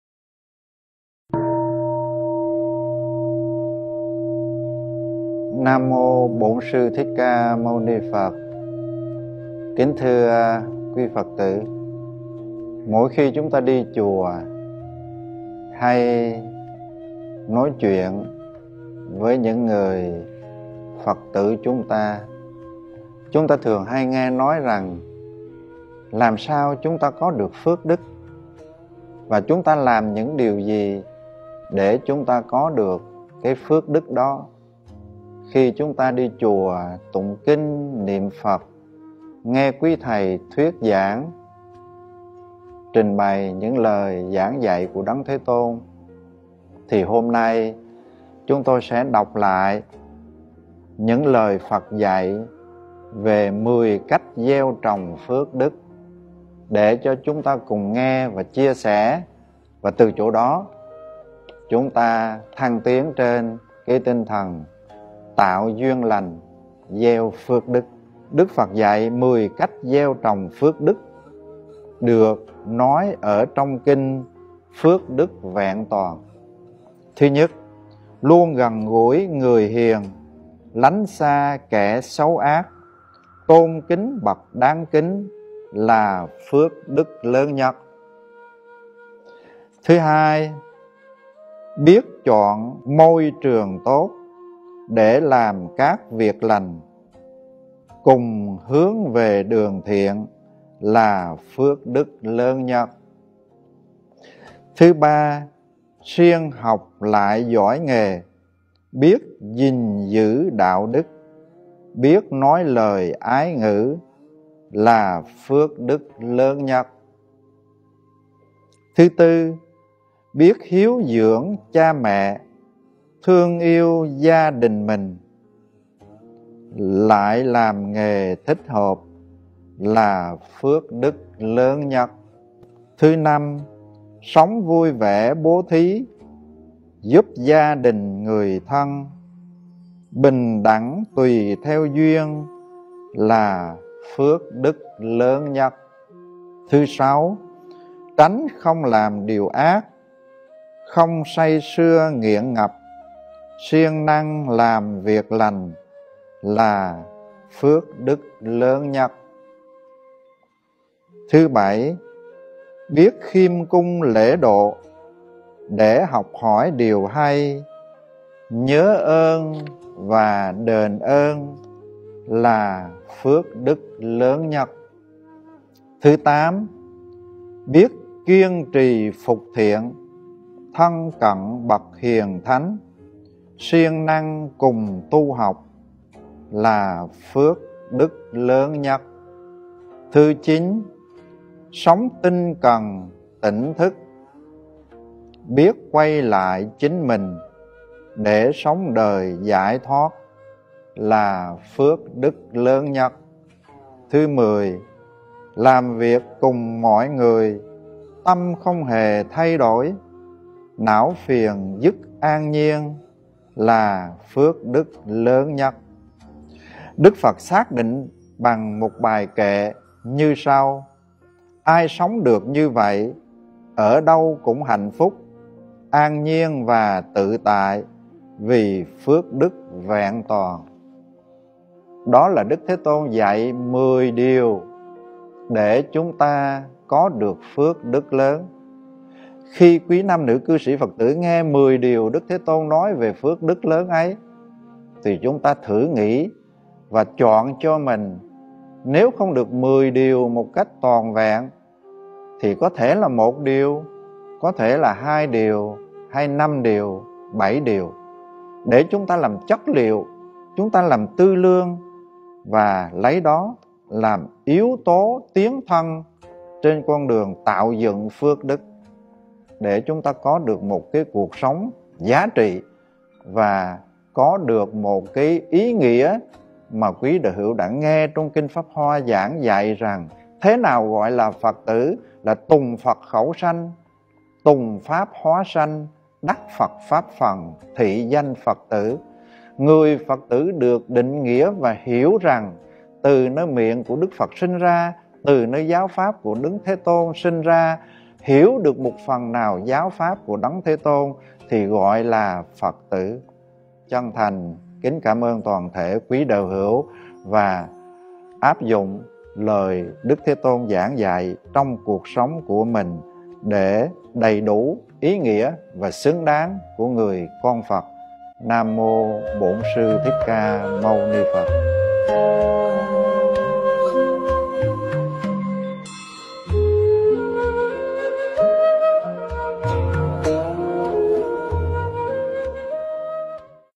5 Phút Phật Pháp: Gieo Trồng Phước Đức - Hòa Thượng Thích Nguyên Siêu * Chùa Phật Đà San Diego
5 Phút Phật Pháp: Gieo Trồng Phước Đức - Hòa Thượng Thích Nguyên Siêu
Chùa Phật Đà San Diego
4655 Ruffner St, Suite 180